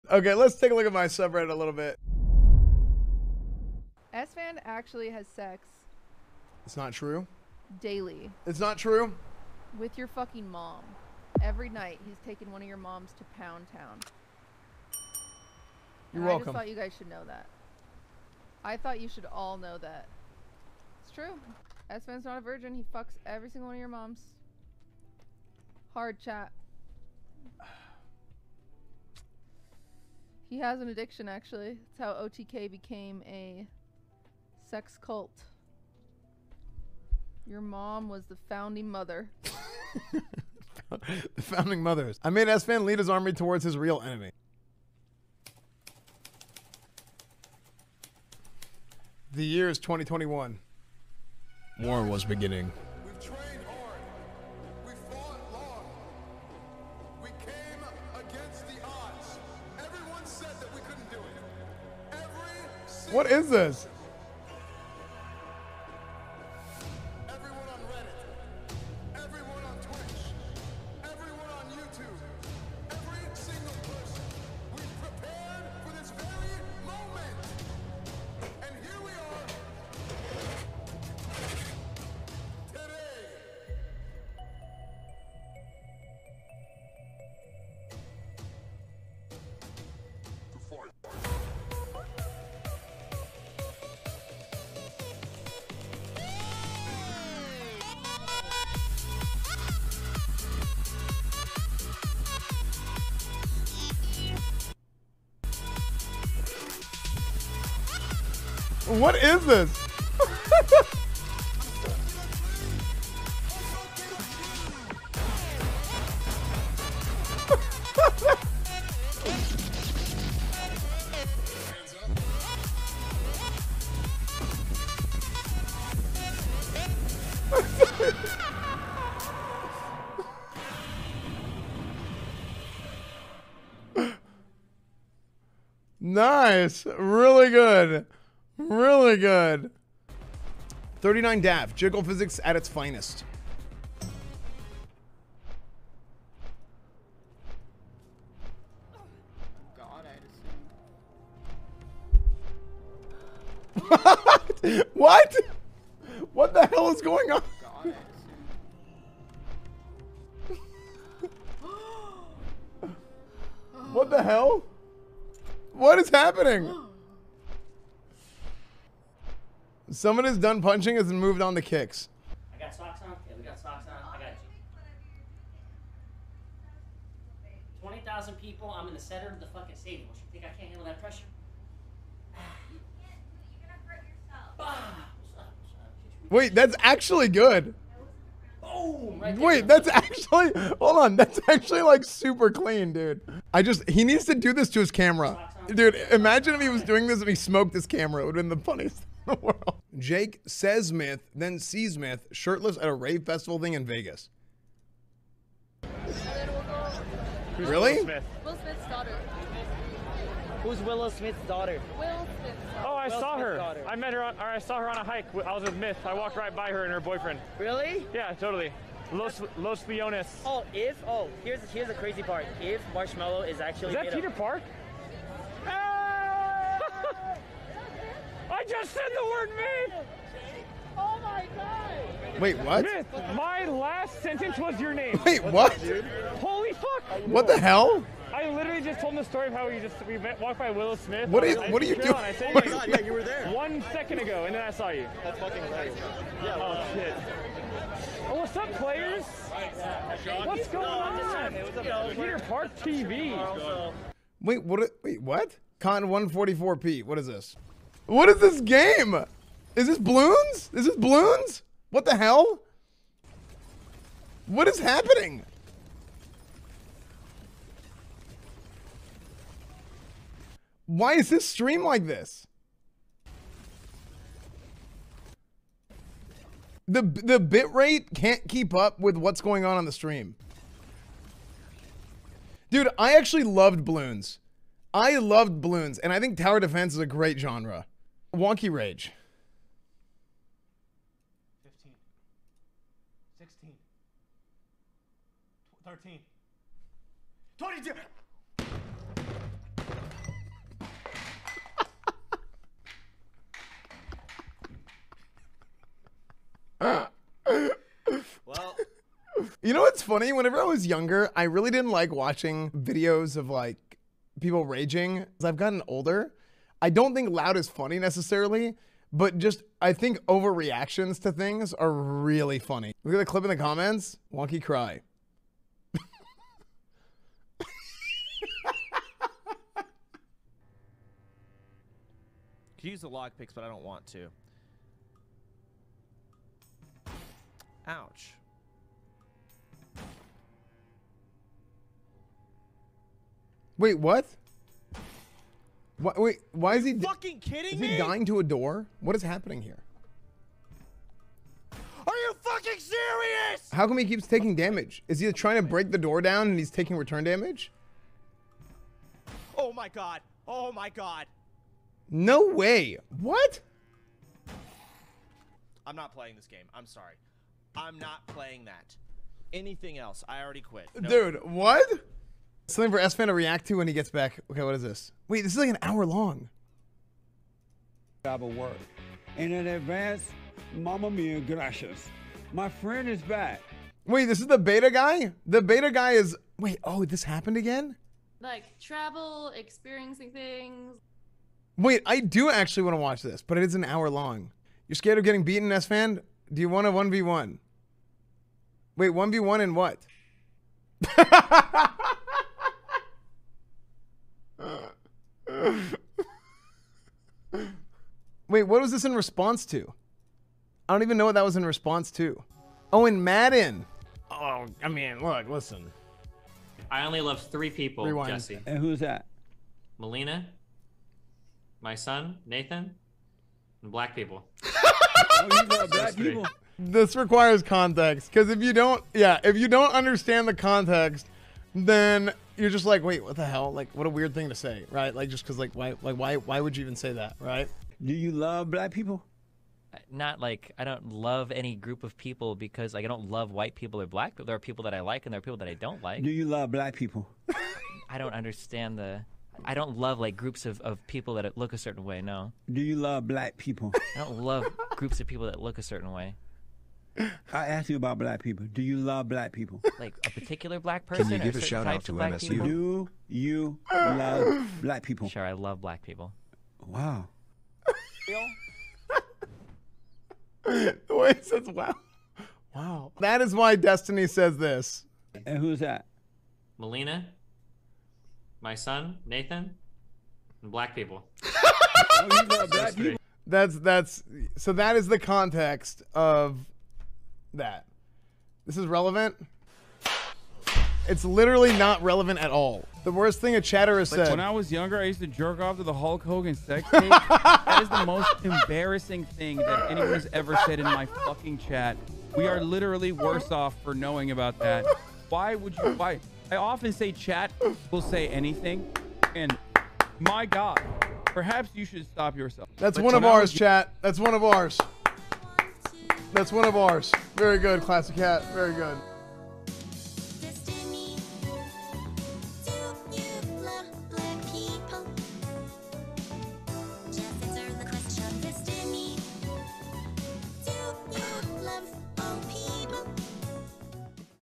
QTCinderella Reveals The Truth About Esfand's Relationship Status | Esfand Reddit Recap
QTCinderella reveals the truth about Esfand's relationship status, and Summit 1g does one of the most impressive stunts Esfand has ever seen!